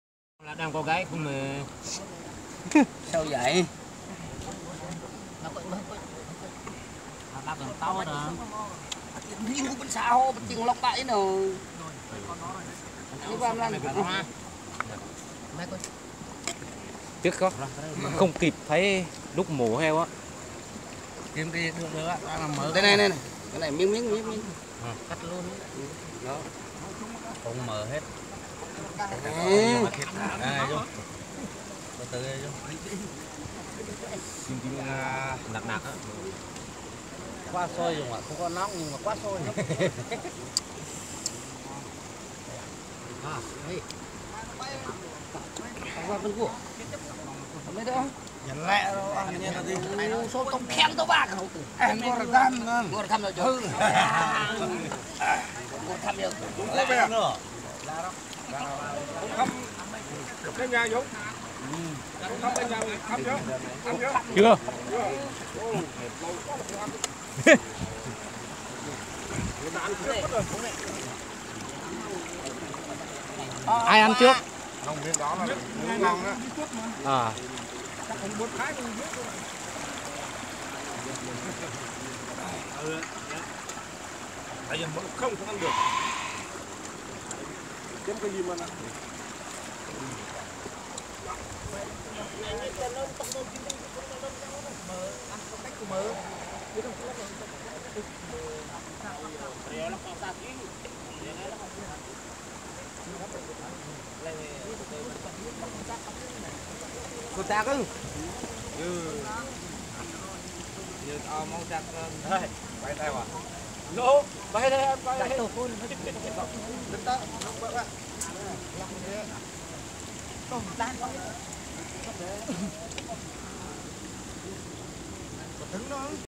ĂN THỊT HEO NÓNG TÂY NGUYÊN,MỔ HEO TẠI SUỐI BÔNG KRANG
ĂN THỊT HEO NÓNG TÂY NGUYÊN , DÂN CHƠI TÂY NGUYÊN, ĂN TẾT
MỔ HEO TẠI SUỐI BÔNG KRANG mời các bạn vào xem